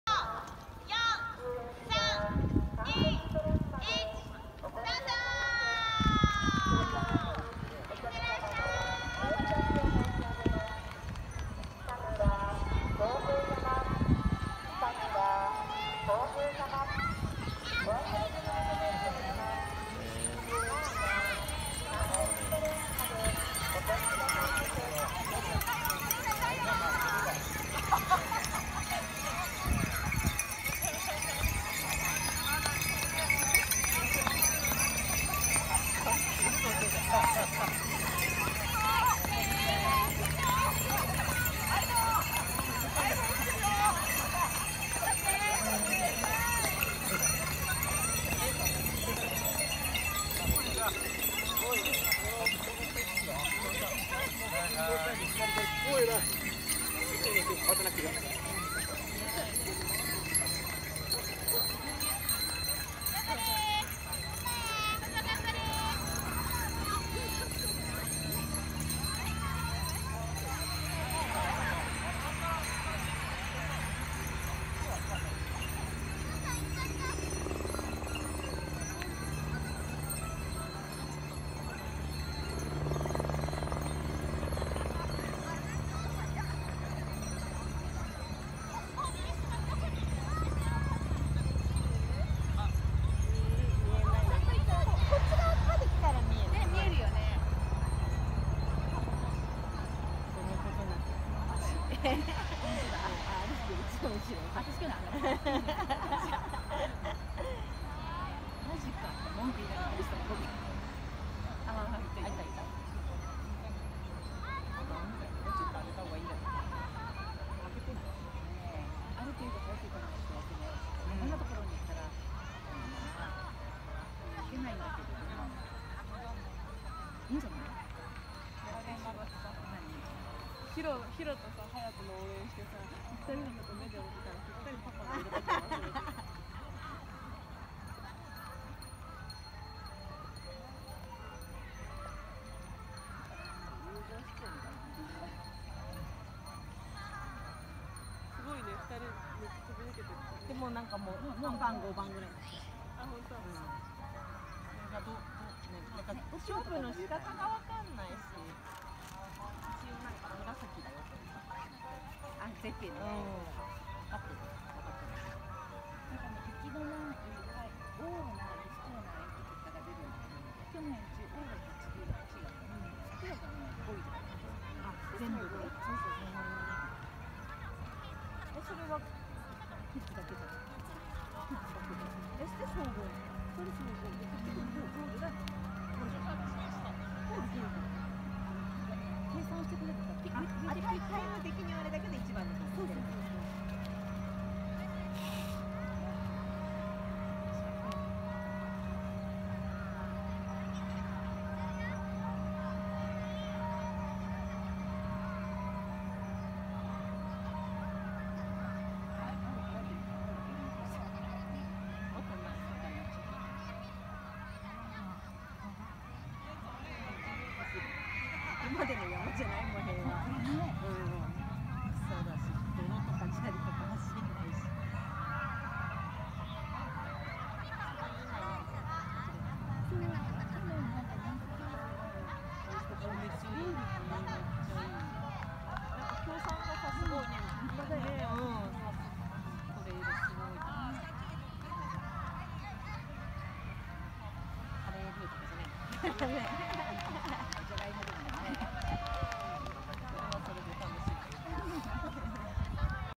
北海道アウトドアフェスティバル2019 inルスツ トレイルランニング②
キッズ、5km、15kmが11時にスタートした動画です。